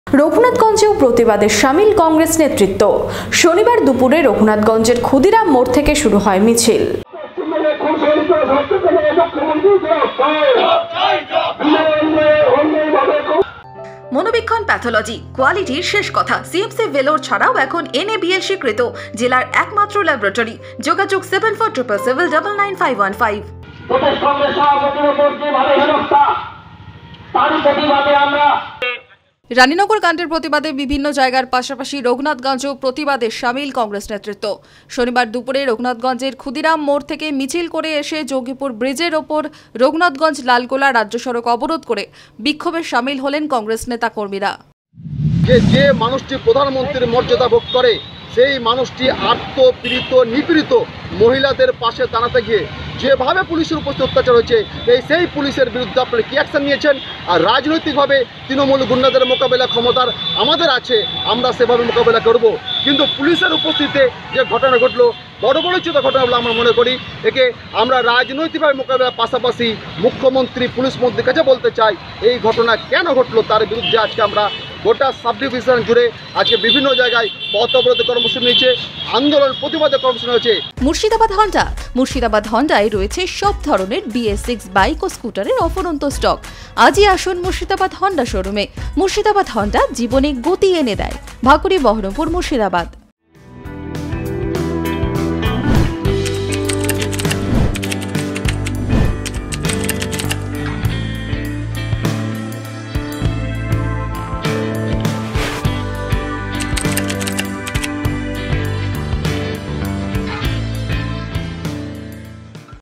রঘুনাথগঞ্জে প্রতিবাদে কংগ্রেস
রানিনগর কাণ্ডের প্রতিবাদে বিভিন্ন যায়গার পাশাপাশি রঘুনাথগঞ্জেও প্রতিবাদে সামিল কংগ্রেস নেতৃত্ব। শনিবার দুপুরে রঘুনাথগঞ্জের ক্ষুদিরাম মোড় থেকে মিছিল করে এসে জঙ্গিপুর ব্রিজের উপর রঘুনাথগঞ্জ লালগোলা রাজ্য সড়ক অবরোধ করে বিক্ষোভে সামিল হন কংগ্রেস নেতা কর্মীরা।

Madhyabanga News